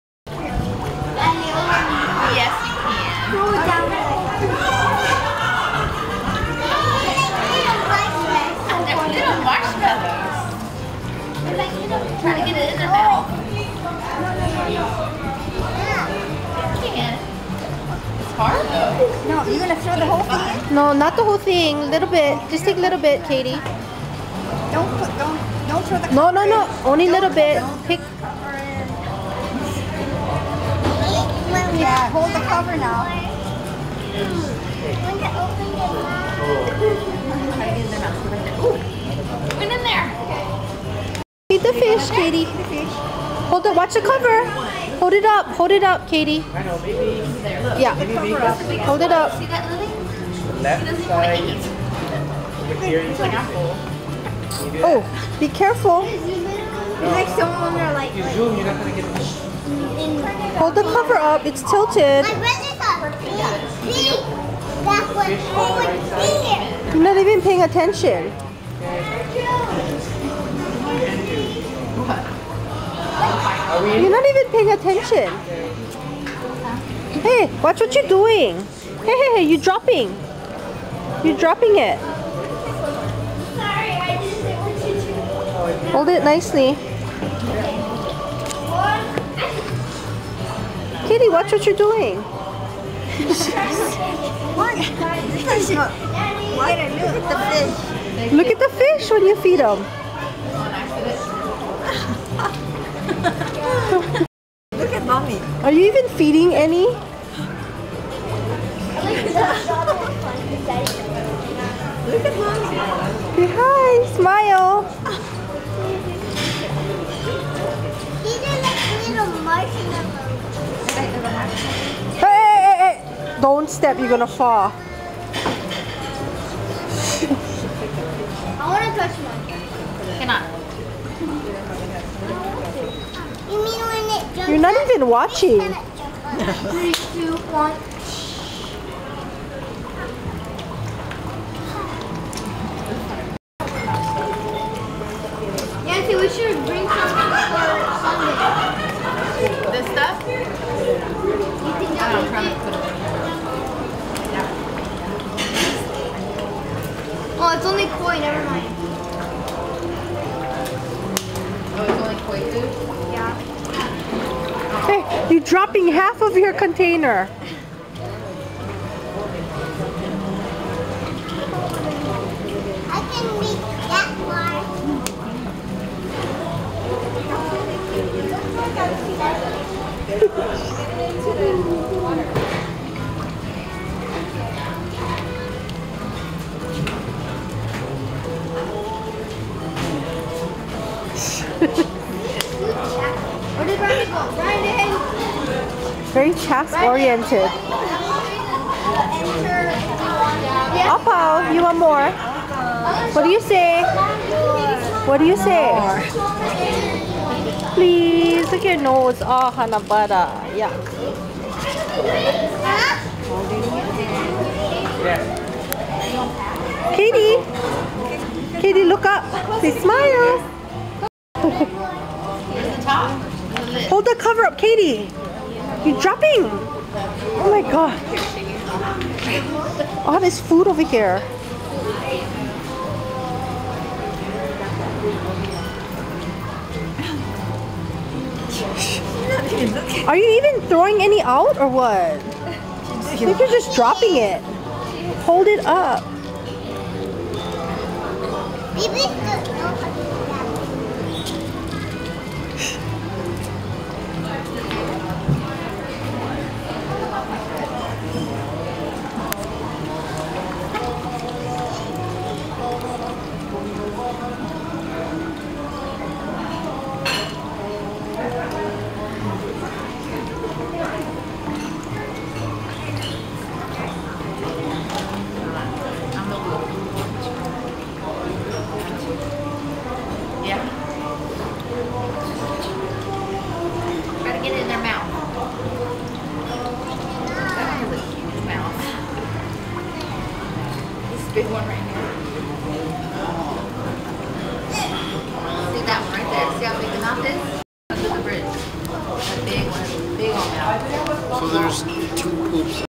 28Jan2012 Cadie feeding the koi at Pagoda CIMG1413
Watch how attentive Cadie is to feeding the fish.  She had fun and kept going to papa to buy her more fish food.